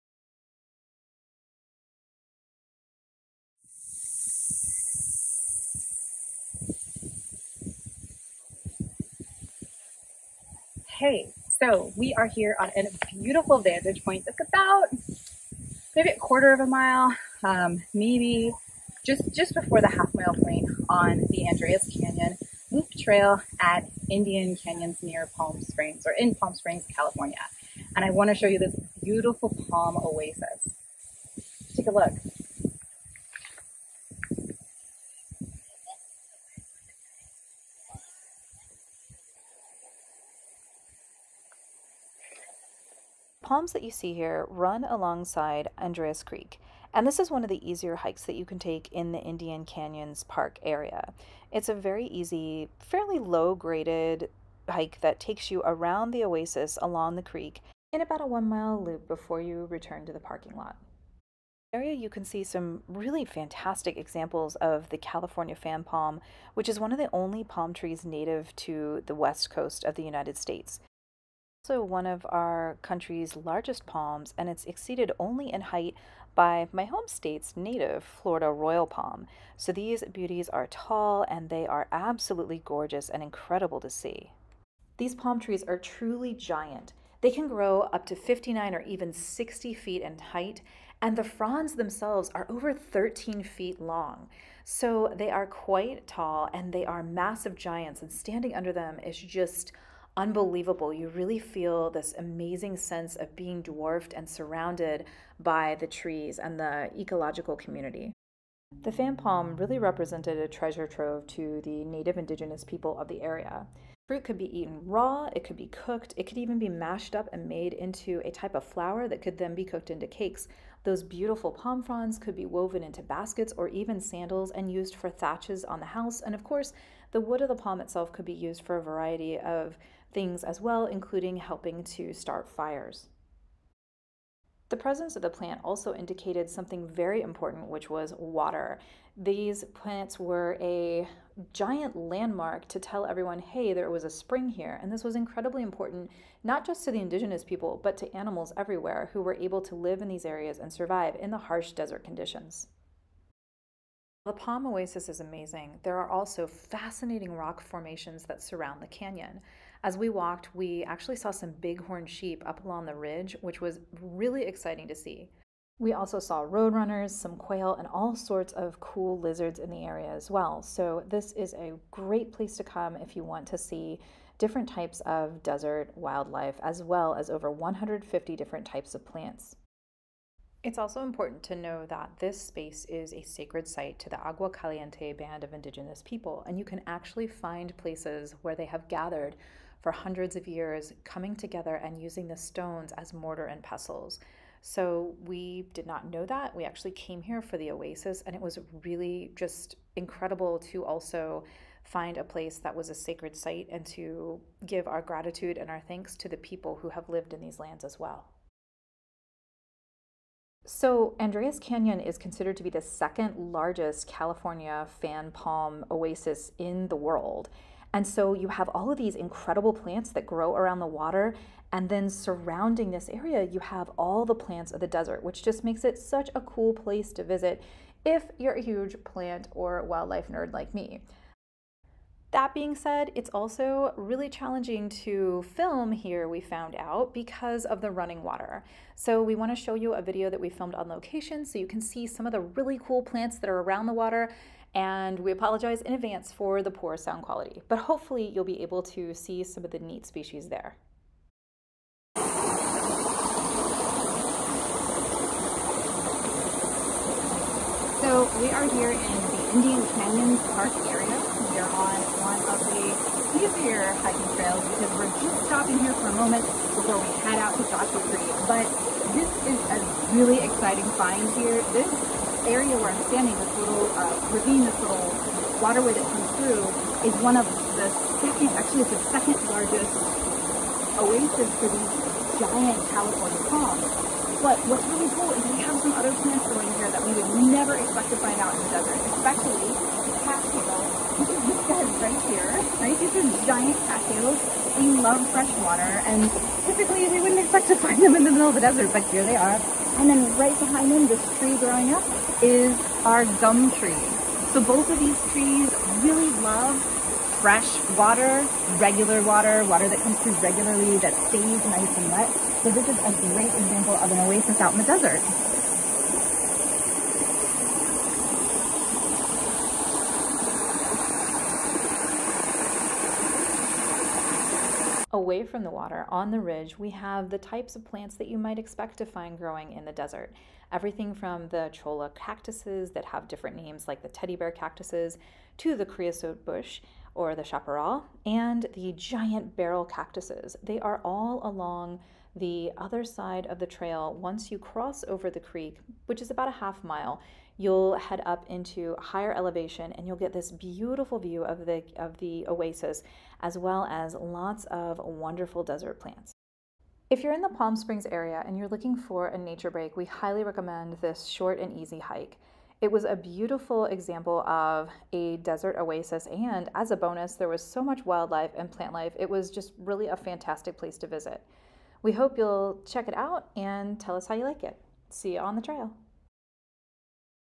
Exploring Andreas Canyon in Palm Springs & Meeting California Fan Palms w Registered Herbalist Misha
Join Misha and Dr. Brian as they explore Andreas Canyon in Palm Springs. This short video gives an overview of the location and will introduce you to one of the largest palm species in North America: The California Fan Palm